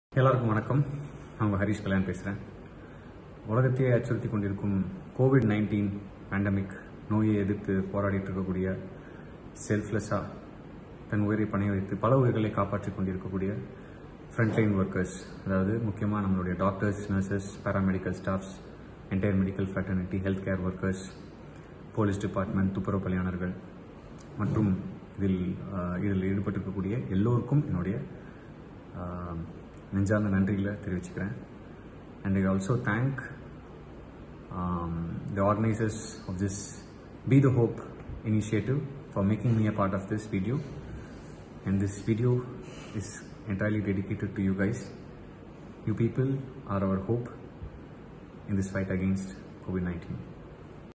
Harish Kalyan thanks all the Front-line warriors of COVID-19
Harish Kalyan, joins us in thanking all the frontline warriors of COVID-19.